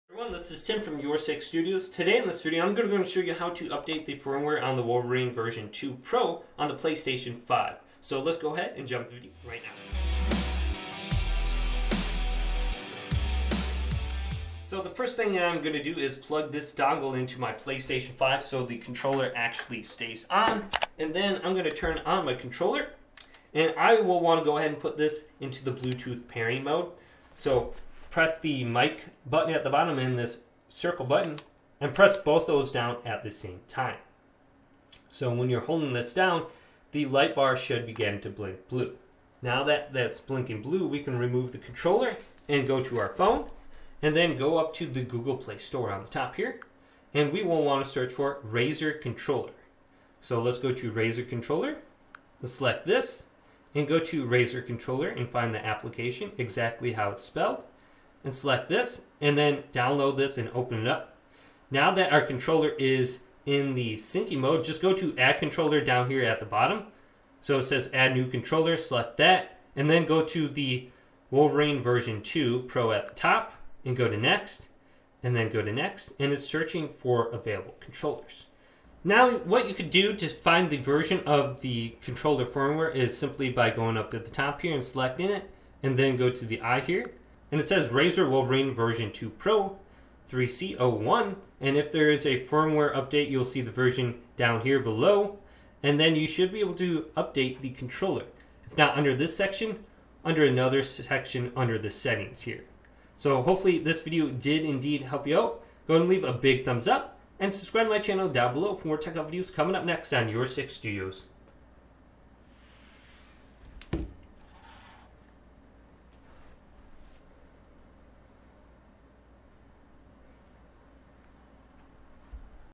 How to Update Firmware on Wolverine V2 Pro PS5 Controller from Razer (Fast Tutorial)
Do you want to know how to update the firmware on the Wolverine V2 Pro controller on the PS5! This razer controller has a mobile app called Razer controller that allows you to update the firmware on the controller. As of 1 / 7/ 2023 there is no update for deadzone sliders, however, hopefully with a firmware update in the future you can adjust the deadzones on this controller.